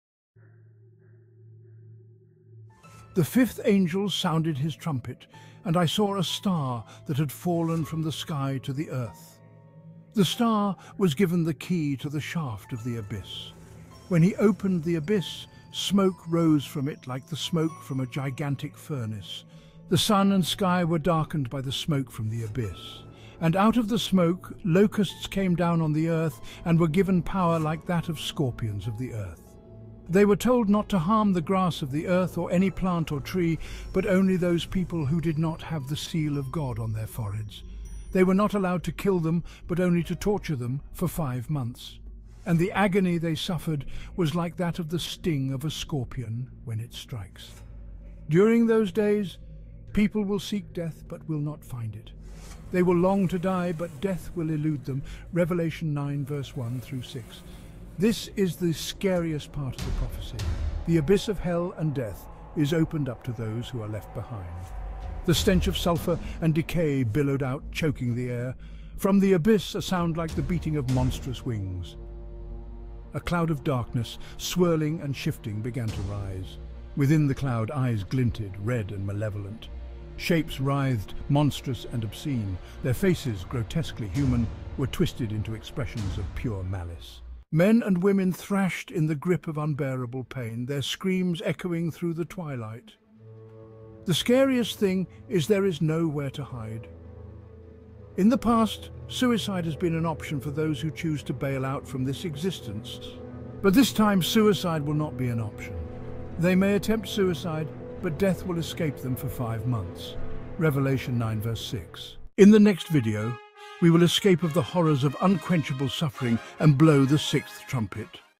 The Fifth Trumpet Revelation 9
At the fifth trumpet, the pit to hell will be opened. And its minions will torture those left behind for 5 months without relief even from death. andrewtheprophet @andrewtheprophet @theprophecynews